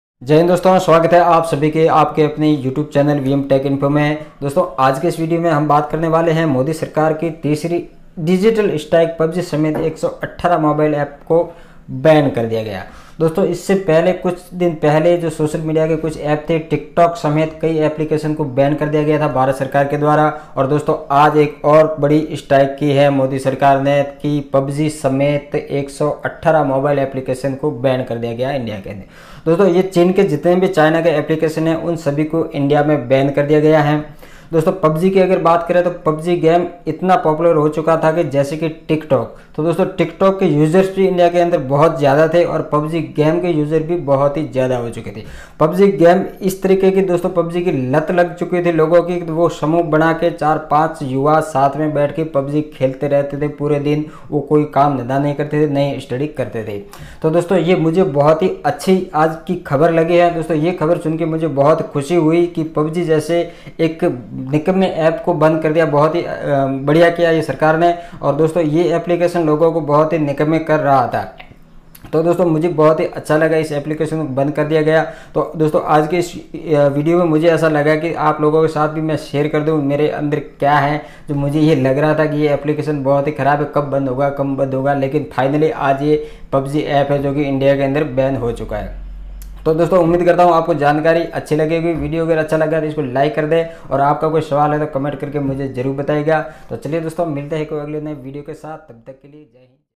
PUBG Banned in India।। 118 app ben।। PUBG का गेम ओवर।।
PUBG का गेम हुआ ओवर अब कई बच्चो का भविष्य होगा सुनहरा। दोस्तो इस गेम का नशा इतना खतरनाक था। जिसकी हम कल्पना भी नही कर सकते है। मोदी सरकार को बहुत बहुत बधाई ऐसे निर्णय लेने पर। 💐💐💐